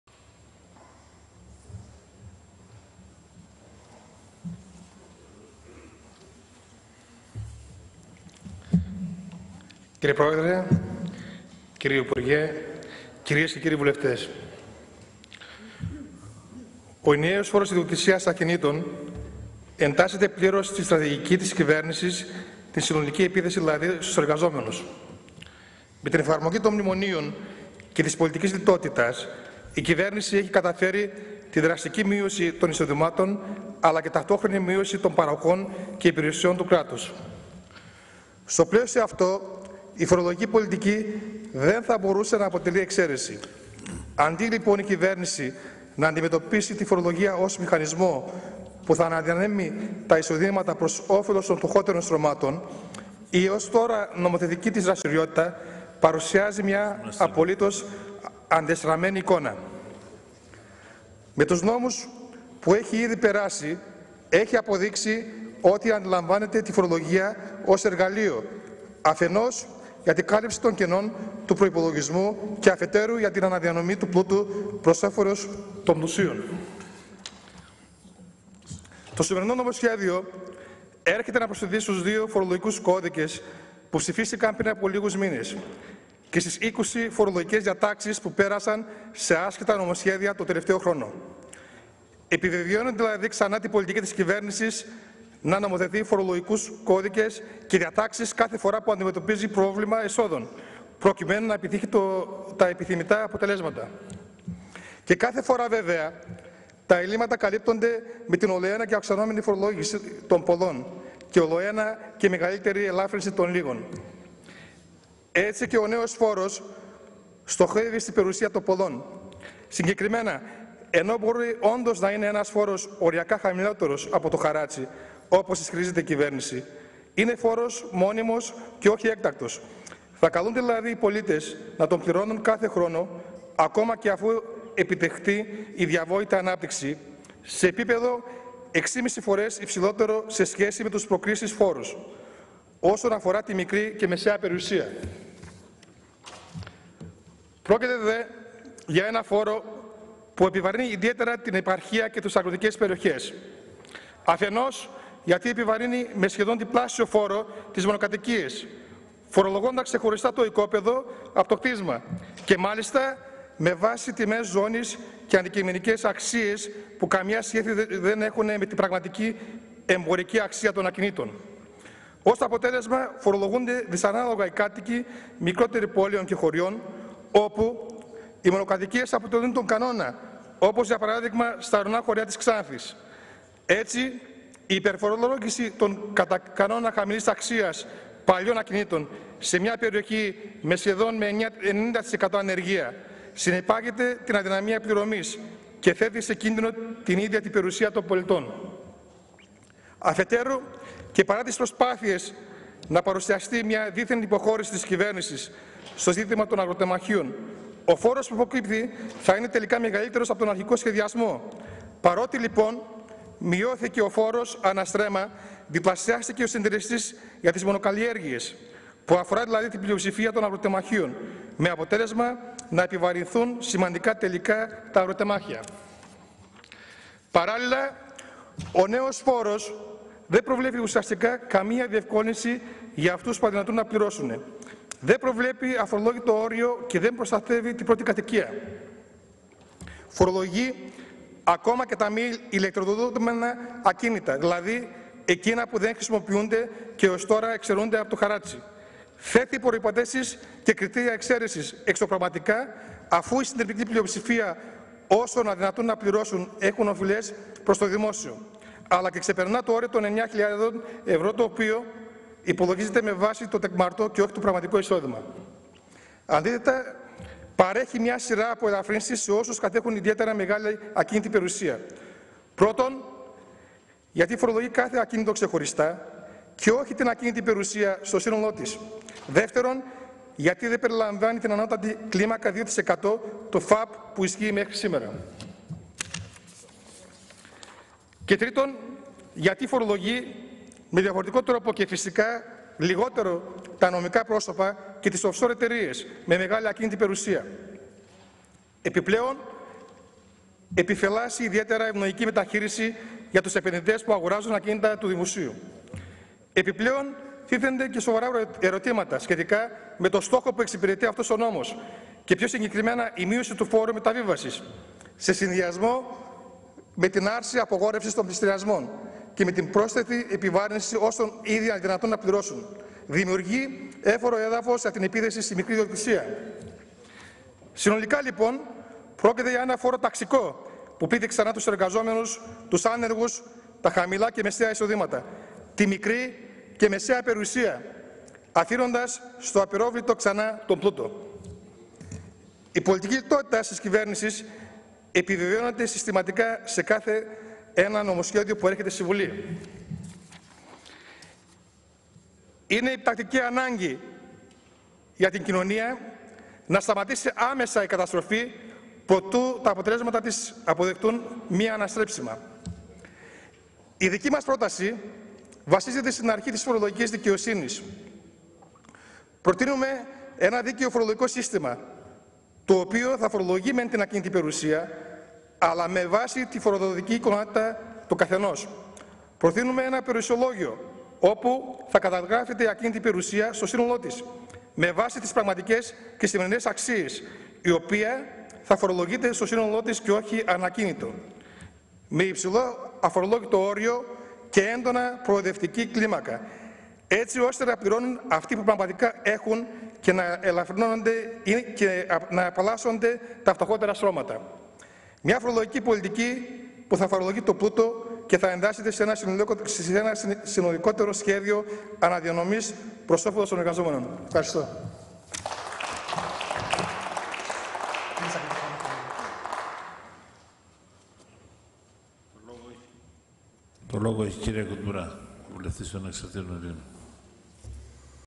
Χουσεϊν Ζεϊμπεκ: Ο ενιαίος φόρος για τα ακίνητα μέρος της επίθεσης της κυβέρνησης στους εργαζόμενους
H πρόταση του ΣΥΡΙΖΑ βασίζεται στην αρχή της φορολογικής δικαιοσυνής "Φορολόγηση της ακίνητης περιουσίας με βάση την οικονομική δυνατότητα του καθενός" "Περιουσιολόγιο στο σύνολό της με υψηλό αφορολόγητο όριο" Μια φορολογική πολιτική που θα φορολογεί τον πλούτο"

Eπεξεργασία: Κατερίνα Μπρέγιαννη